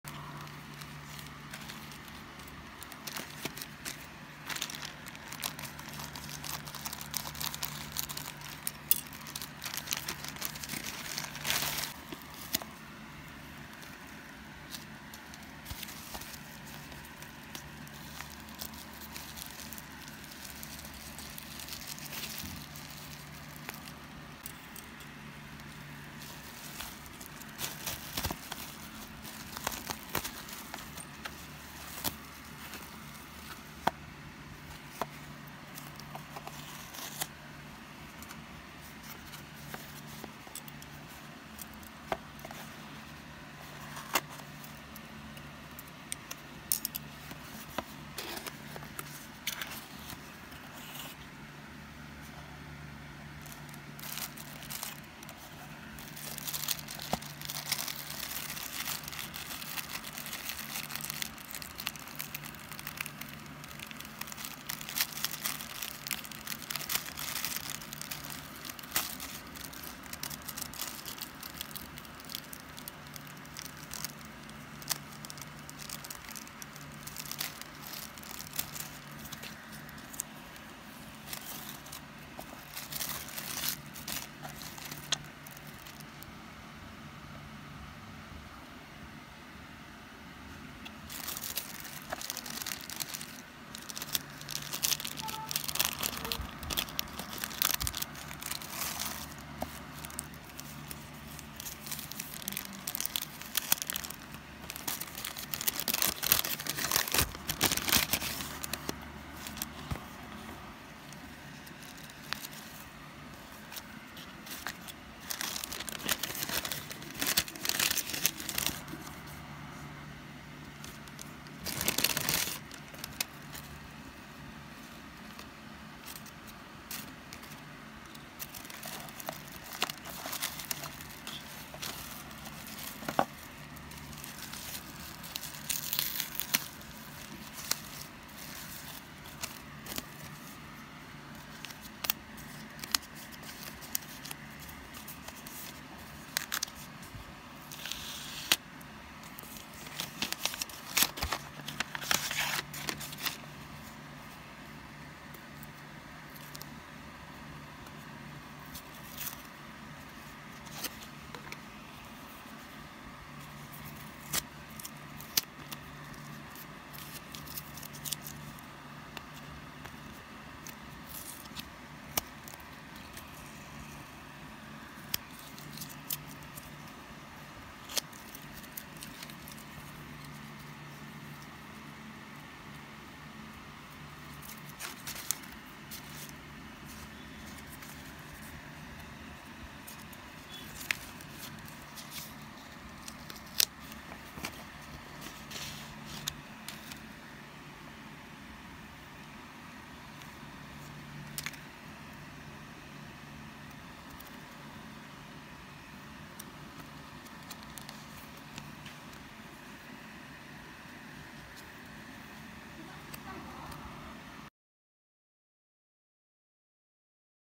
Asmr | unboxed a parcel with me
Hello. I bought a photocard on twt so am going to do an unboxing vid to show you guys what i bought... P.s. this is RAW clip i only edited out my name, number, and address beside thise its all RAW

Gadgets used to film:
7+